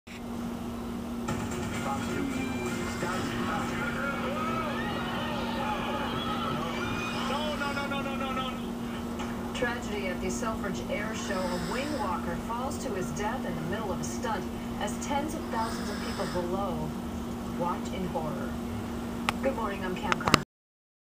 Tragedy Stunt in the Air
Stunt man misses the strap on the helicopter and falls to his death